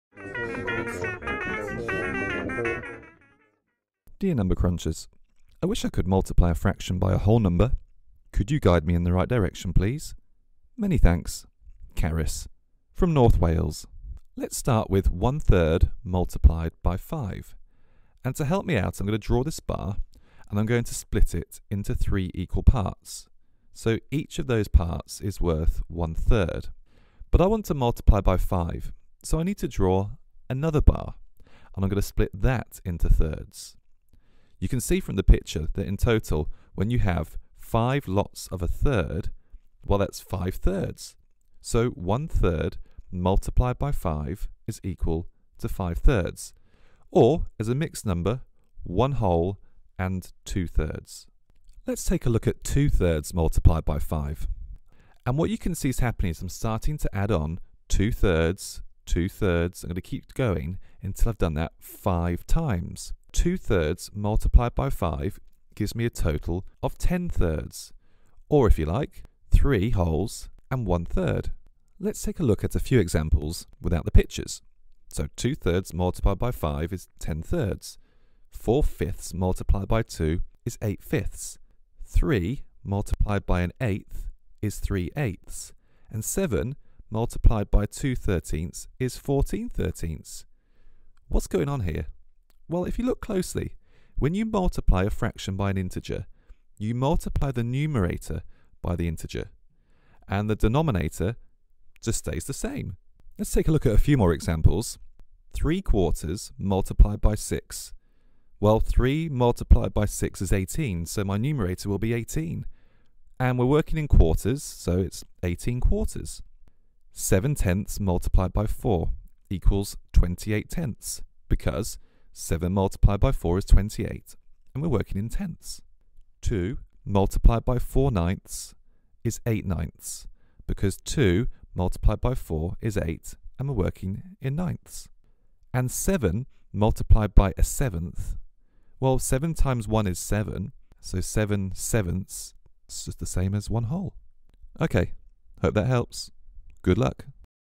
How do you multiply a fraction by an integer? - Ask the Number Crunchers
In this video, Carys from North Wales asks how to multiply a fraction by an integer (whole number).